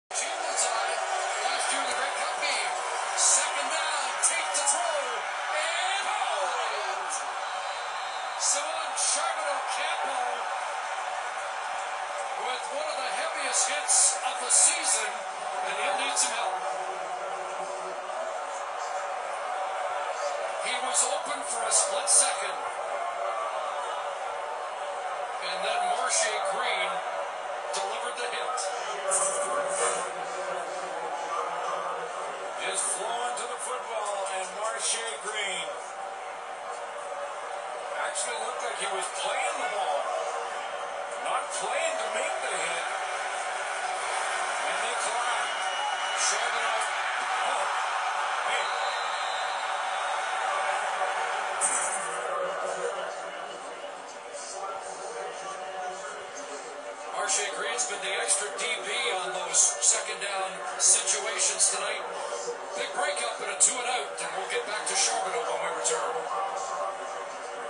Marshay Green big hit on Simon Charbonneau-Campeau
CFL FOOTBALL:  #1 Marshay Green from the Saskatchewan Roughriders makes a big hit on #87 Simon Charbonneau-Campeau form the Calgary Stampeders and takes him out.